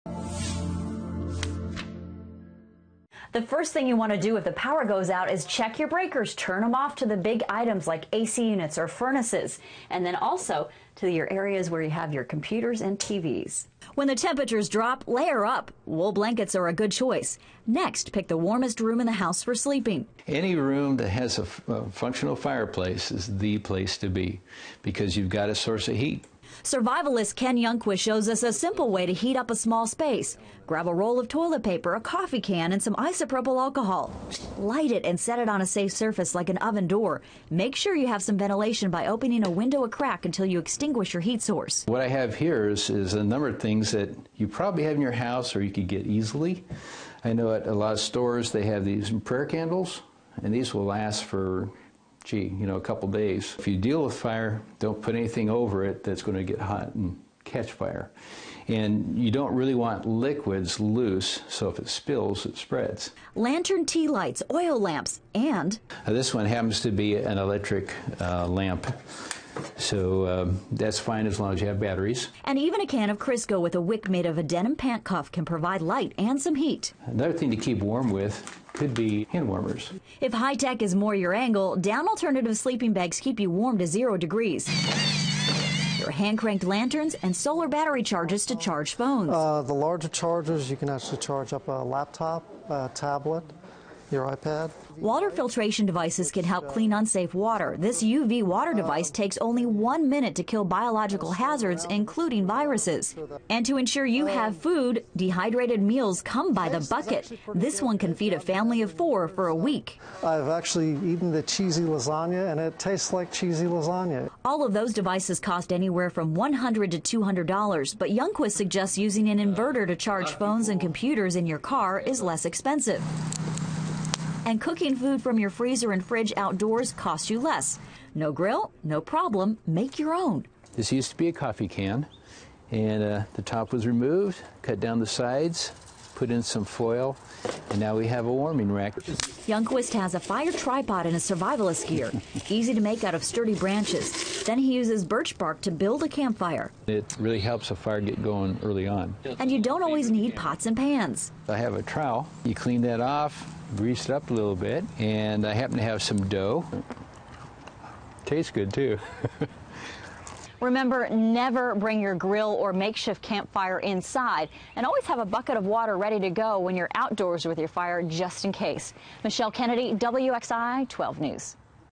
When the power goes out: Survival tips
Losing power for a night isn't so bad, but what if we have a big winter storm that knocks out power for days in the Triad? Are you prepared? | htGreat Outdoor Provision | Ken Youngquist